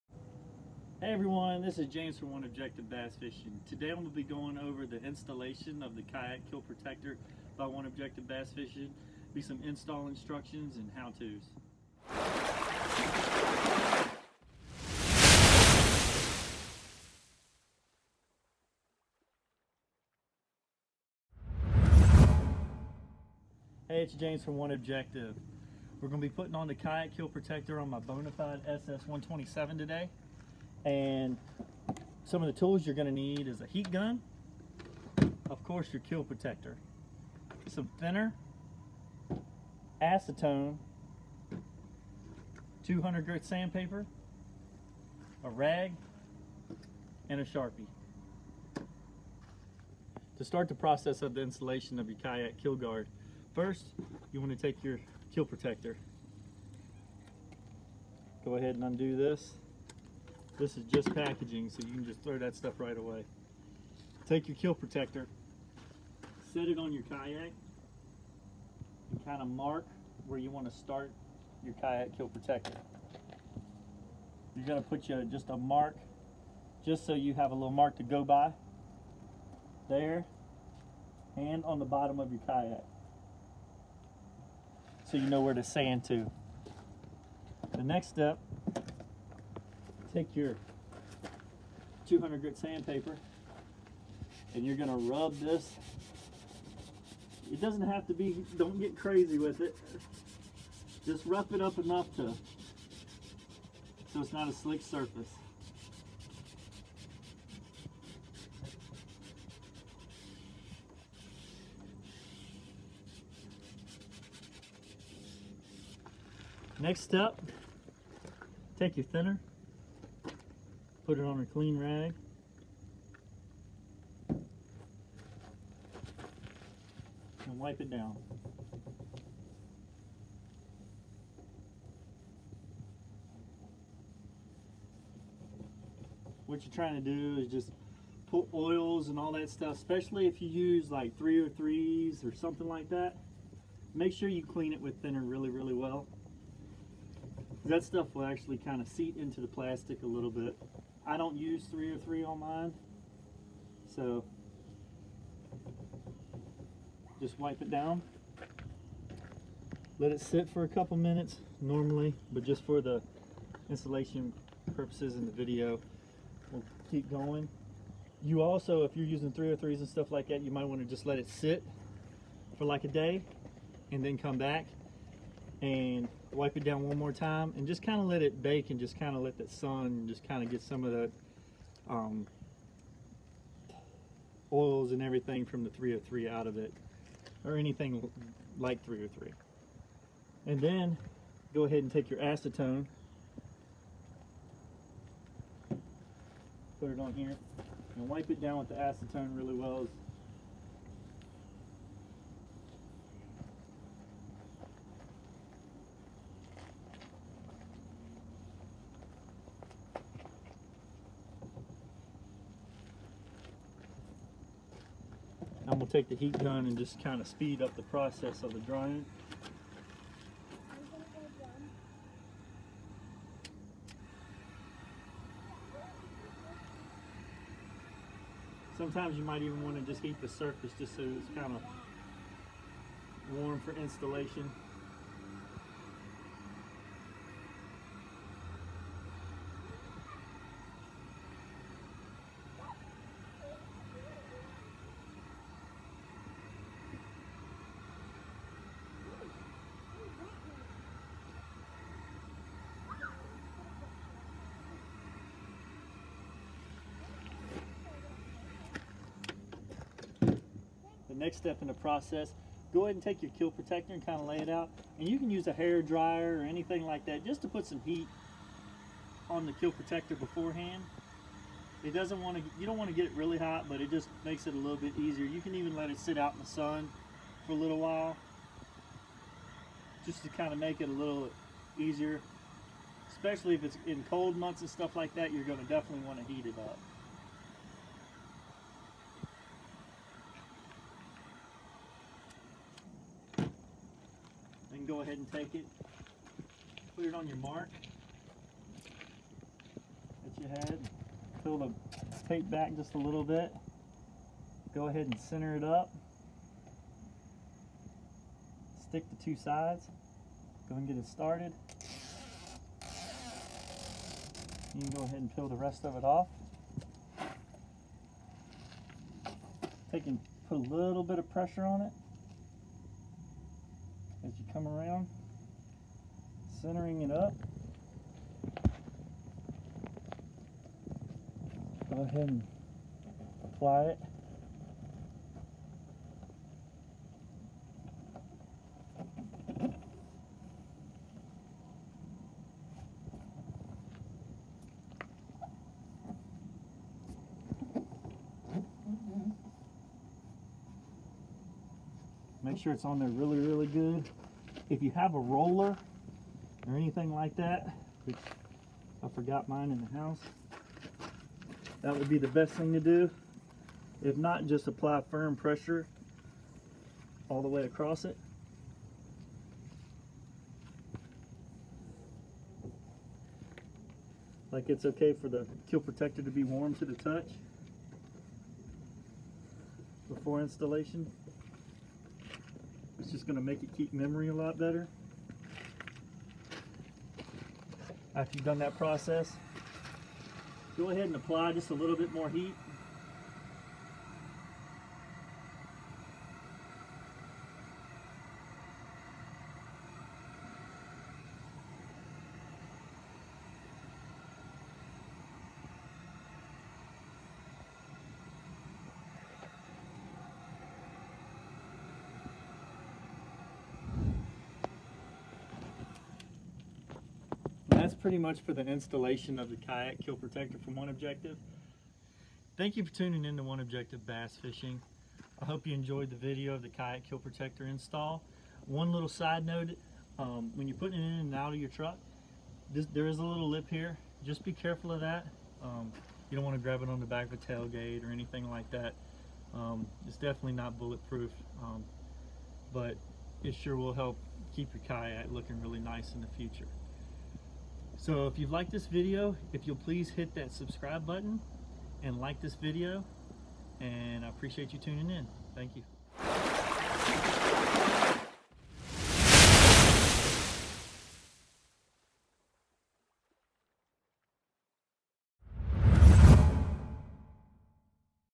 Kayak Keel Protector Install
In this video James goes over how to install the OneObjective Kayak Keel Protector.